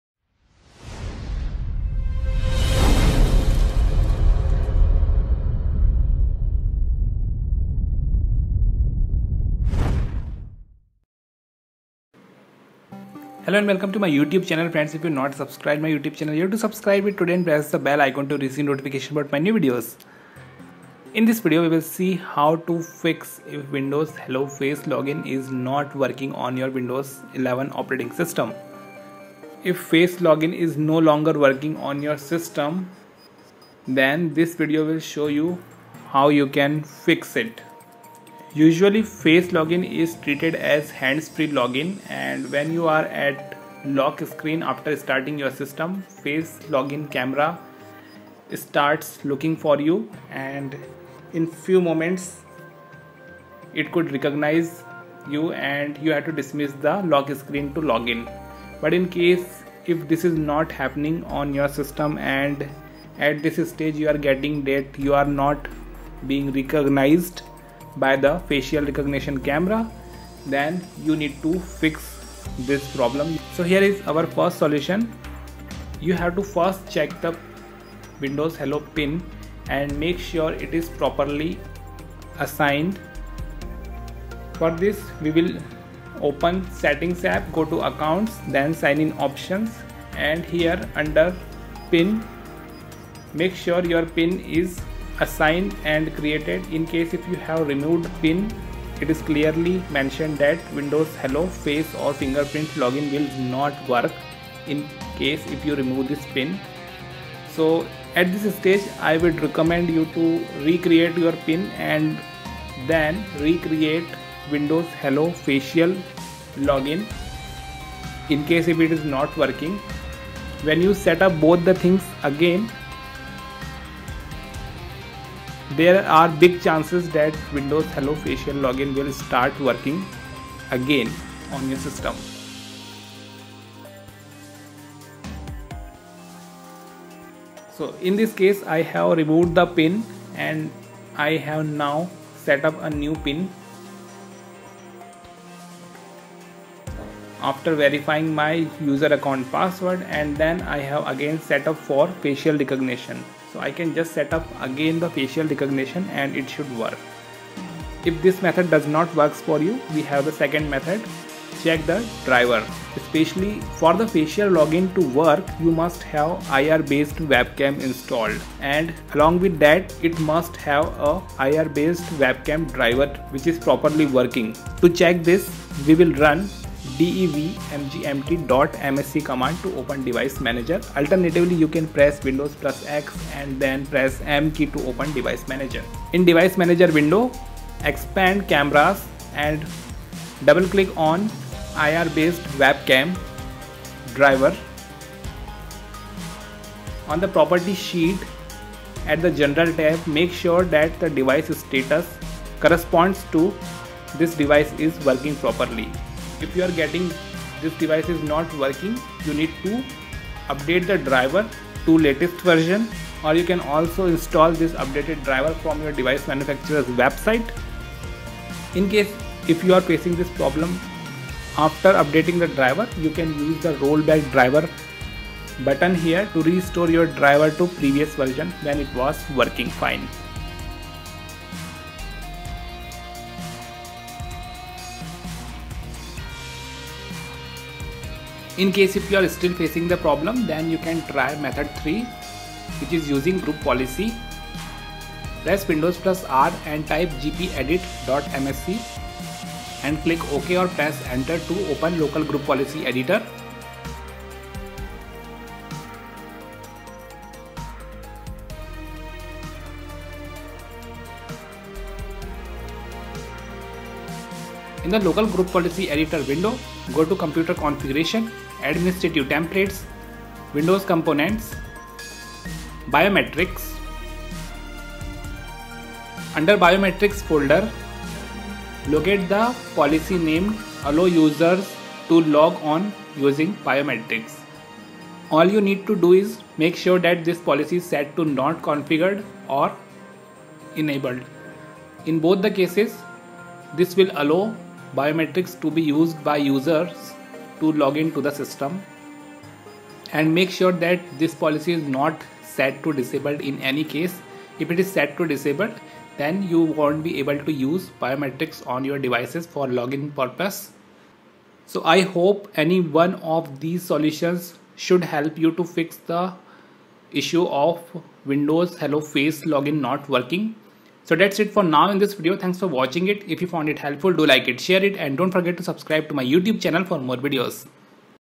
Solved: Windows Hello Face login not working in Windows 11/10
This video will show how to fix Windows Hello Face login not working in Windows 11/10.

Chapters:
00:00 Intro
01:02 Solution 1
02:37 Solution 2
04:00 Solution 3
05:22 End screen